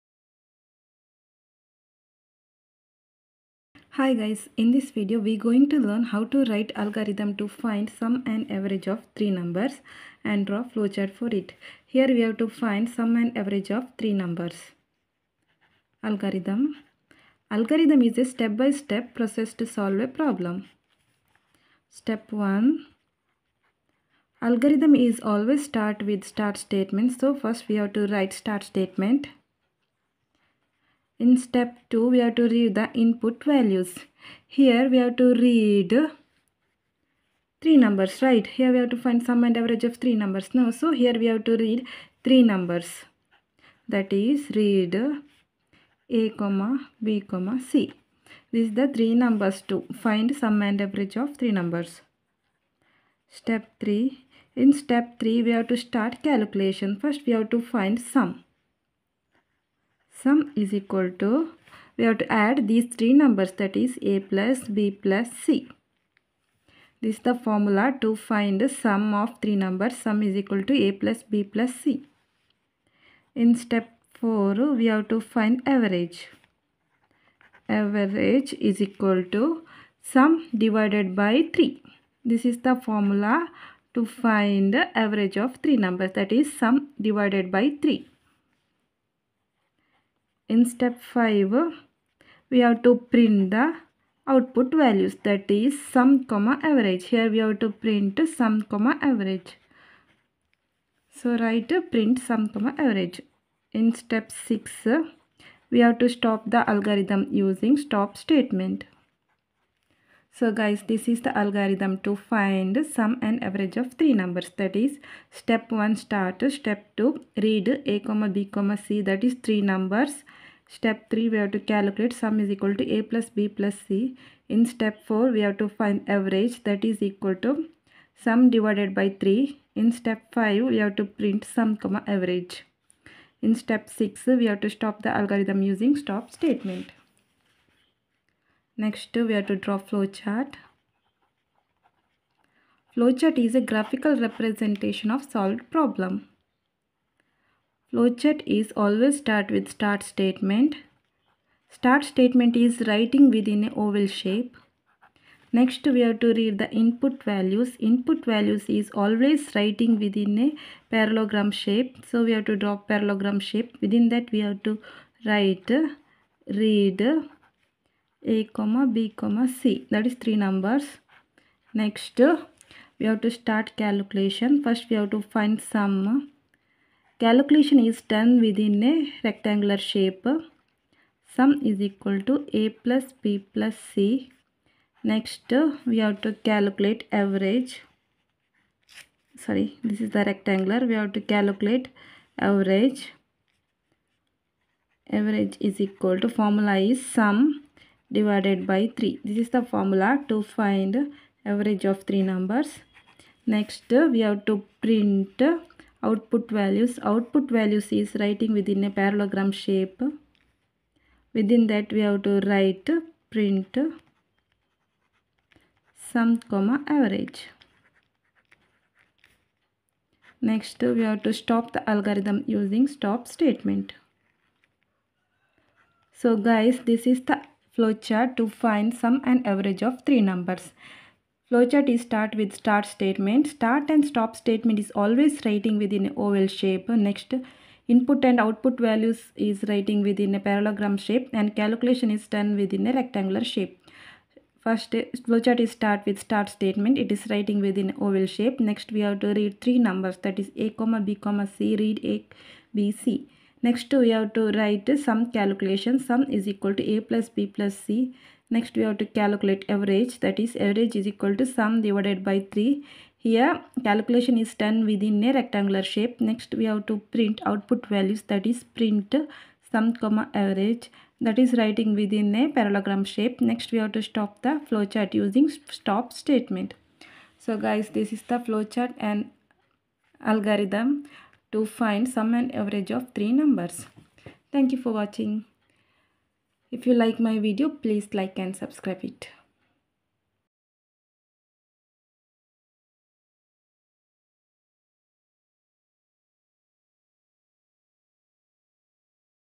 Algorithm to find sum and average of three numbers. Draw Flowchart for it.
Algorithm to find sum and average of three numbers.

Flowchart to find sum and average of three numbers.

Algorithm
Flowchart